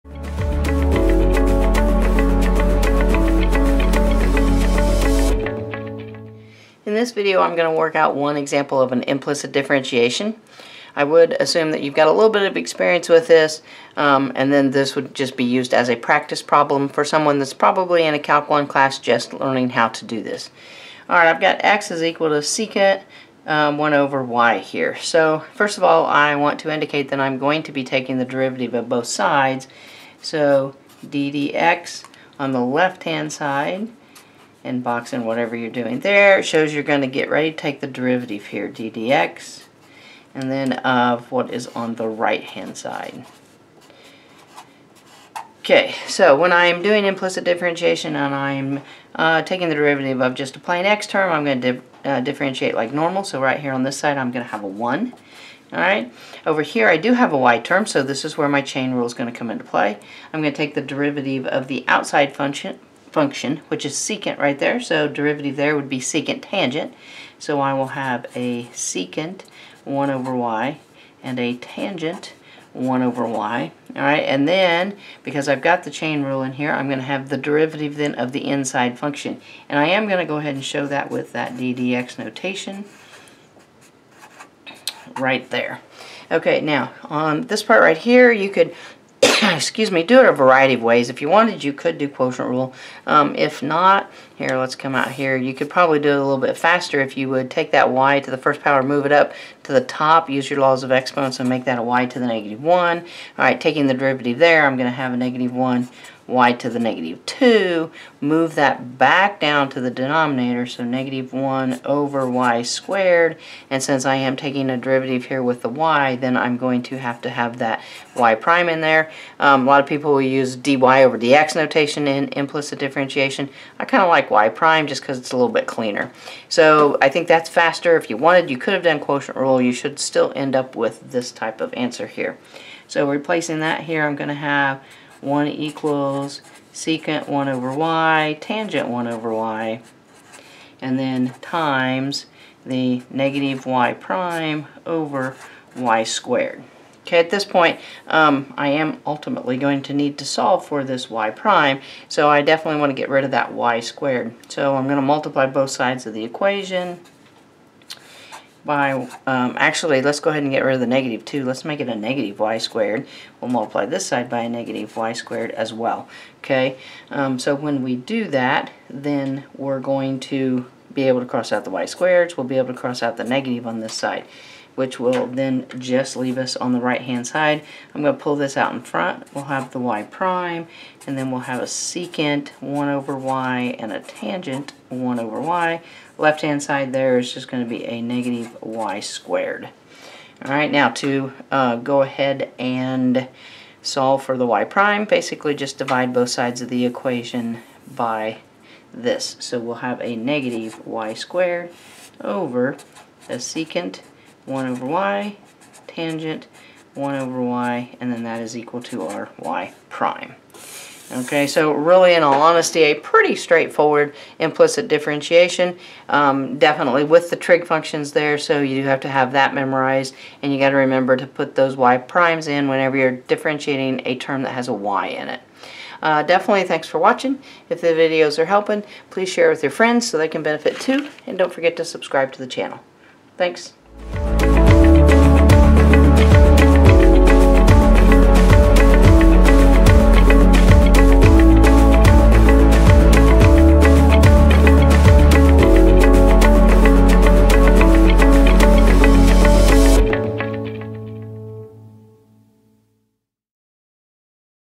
Implicit Differentiation x = sec (1/y) in Calculus 1
This video goes through 1 Implicit Differentiation example of x = sec (1/y).  This type of problem would typically be found in a Calculus 1 class.